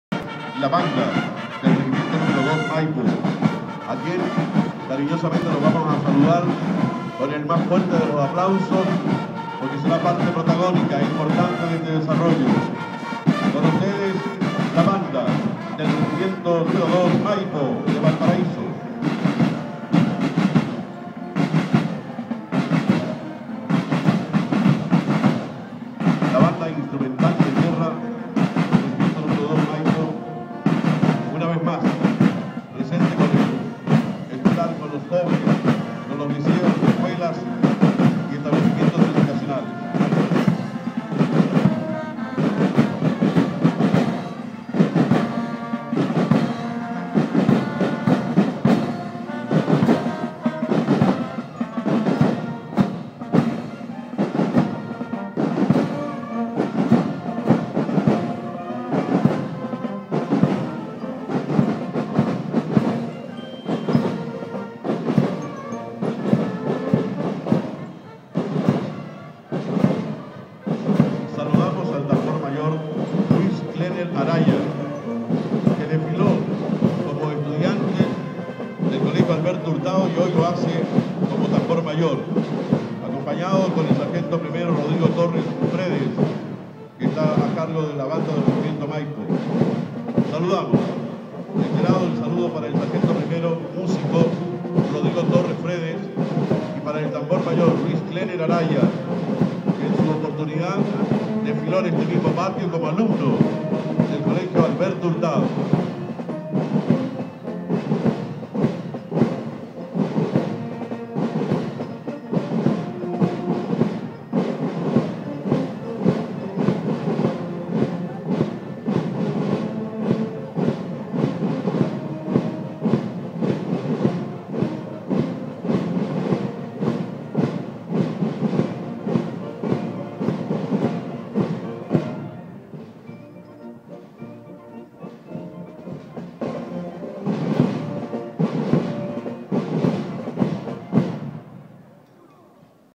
Regimiento de Infantería Motorizada N° 2 Maipo
El día sábado 7 de septiembre se celebro XI Encuentro de Bandas Escolares en el patio de honor del Regimiento Nº 2 Maipo, organizado por el programa "Leven Anclas" de la Radio Congreso...